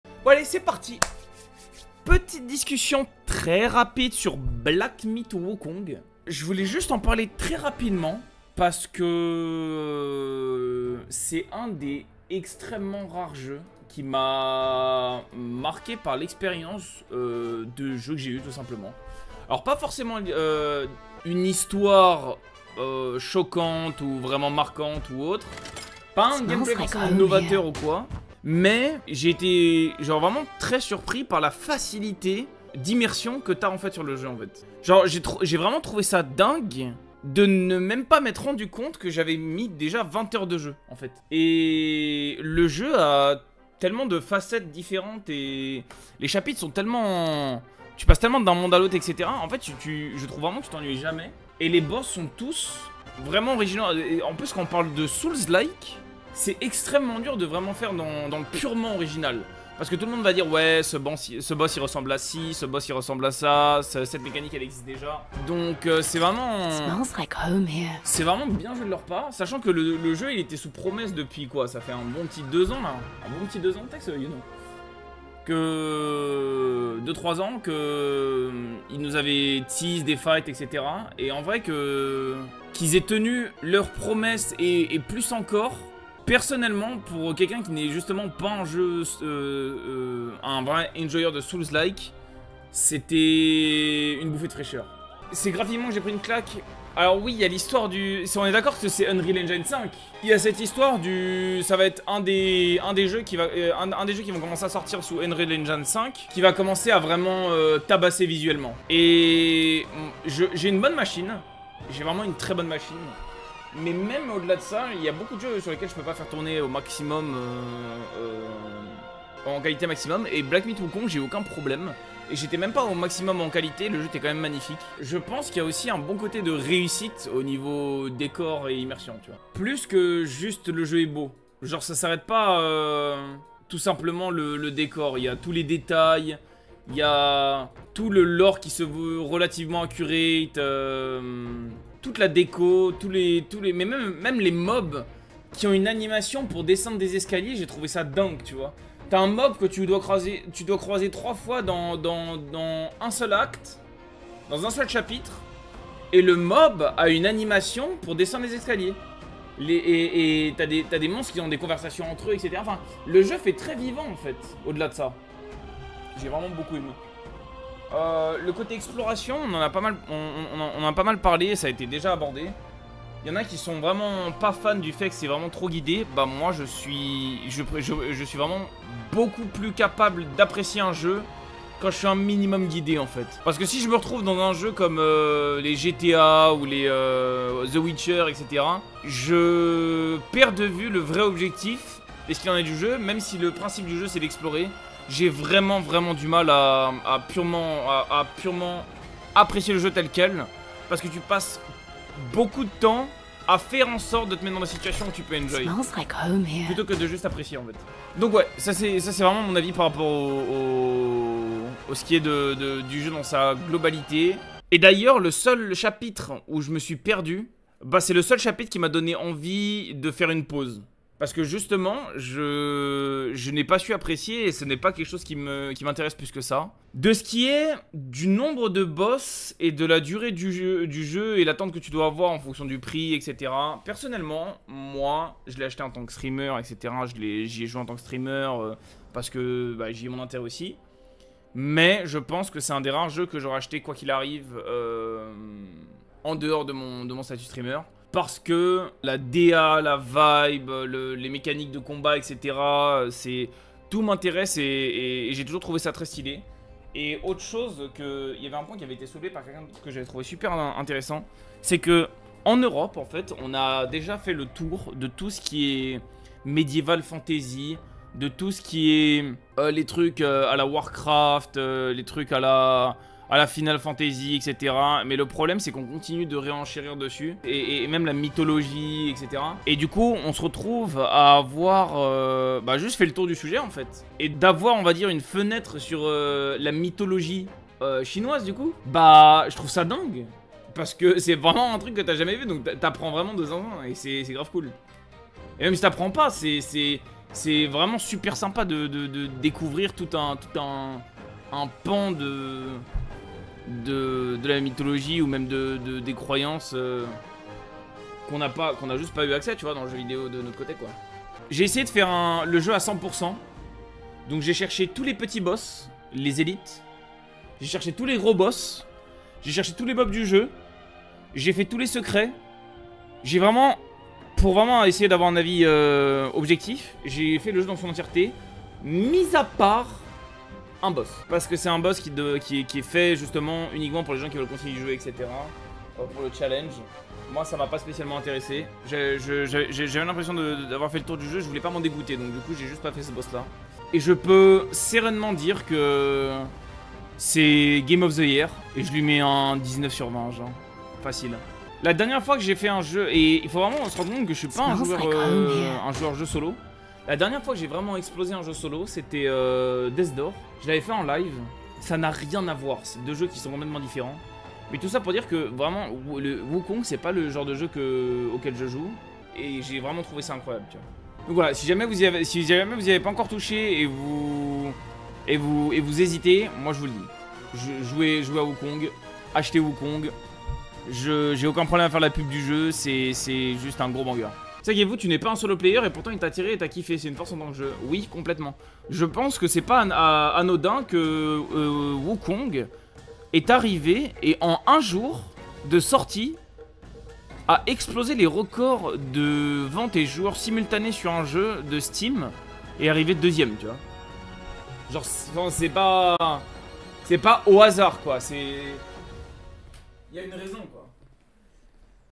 [NO SPOIL] MON AVIS FINAL SUR BLACK MYTH : WUKONG | VAUT-IL VRAIMENT LE COUP? (oui)
Pas l'habitude de faire ça mais on me demandait souvent mon avis et je ressentais aussi le besoin de l'exprimer donc je vous partage ça ici également. En espérant que ça vous plaise :3

Quelques fois*